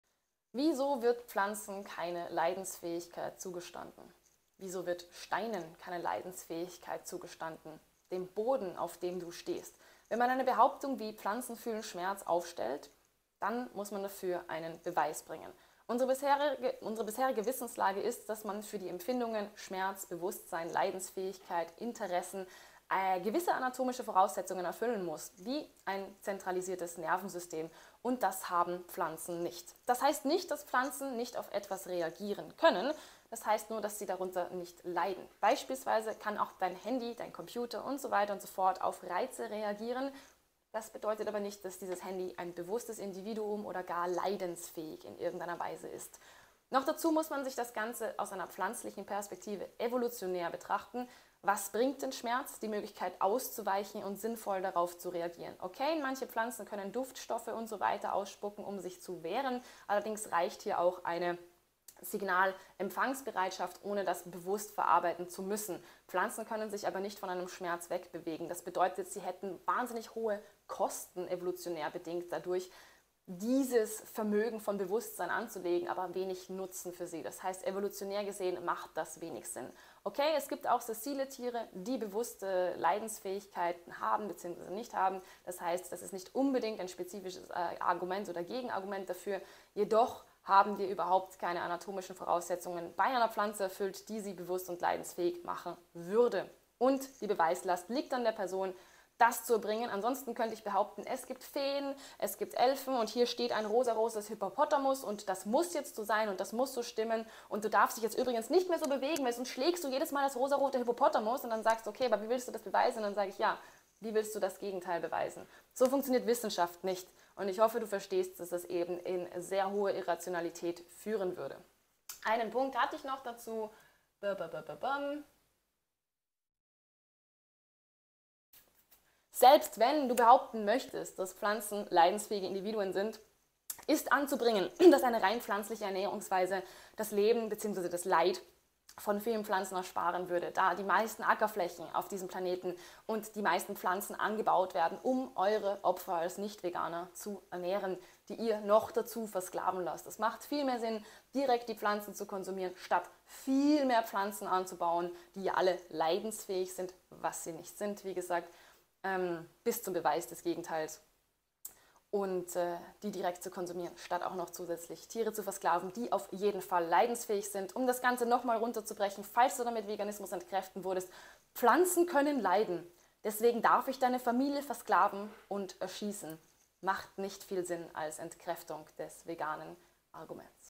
Die Militante Veganerin: Wieso wird Pflanzen keine Leidensfähigkeit zugestanden?
Themenspecial Veganismus mit der Militanten Veganerin (Raffaela Raab) und Felix Olschewski (Urgeschmack)